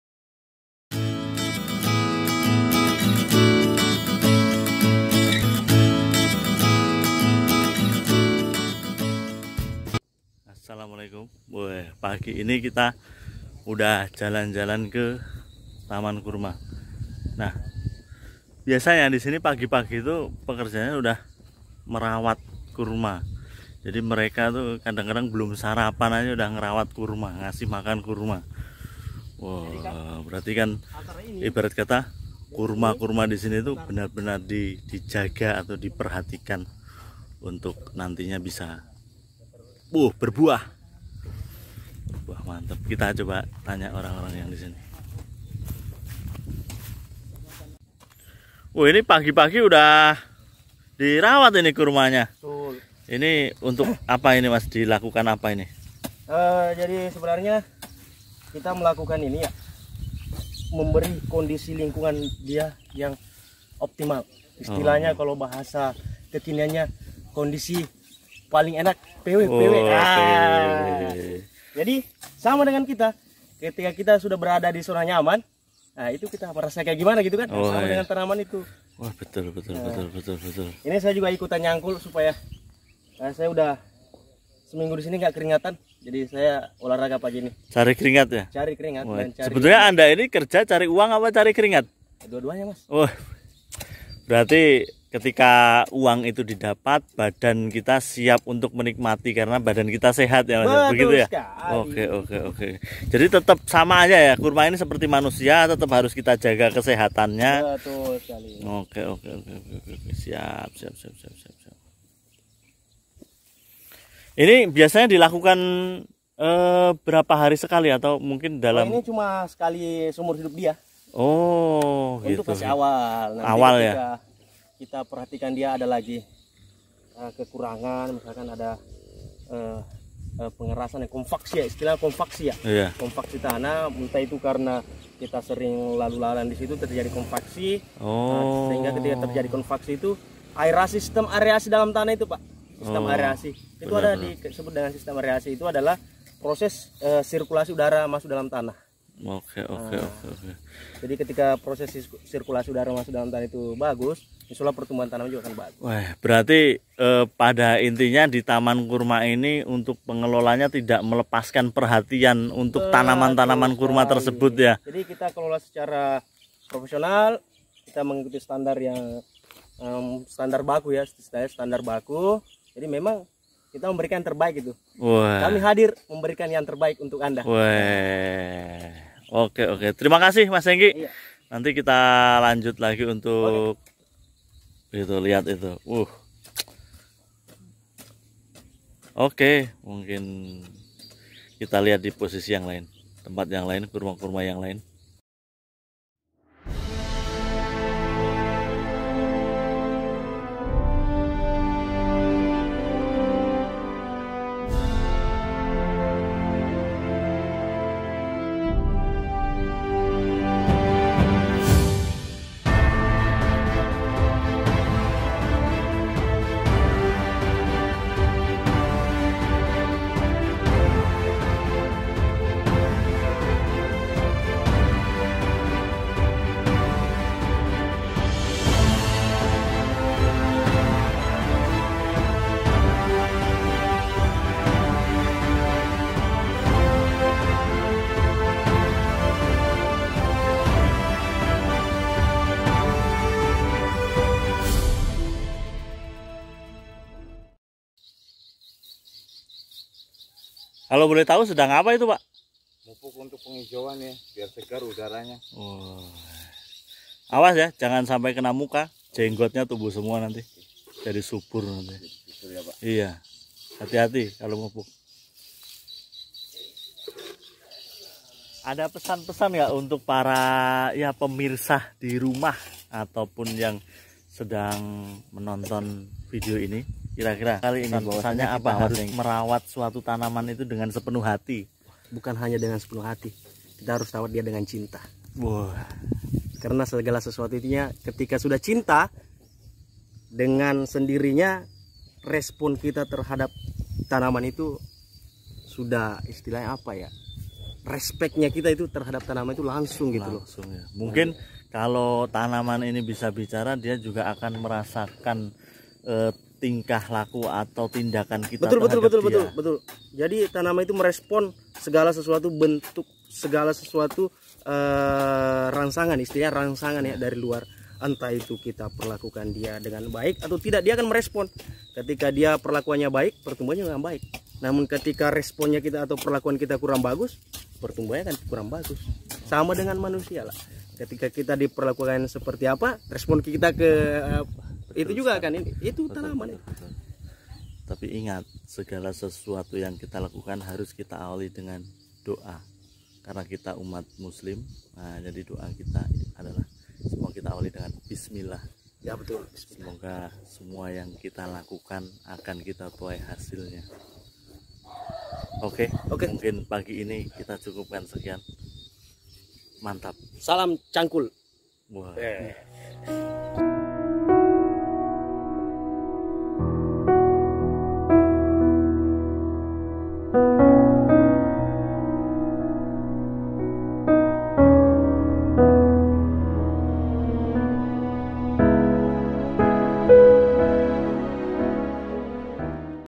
Perawatan KURMA | bersama TAMAN KURMA SAMBOJA
Perawatan diberikan sebaik mungkin demi pertumbuhan dan pembungaan pohon2 kurma di kawasan TAMAN KURMA SAMBOJA...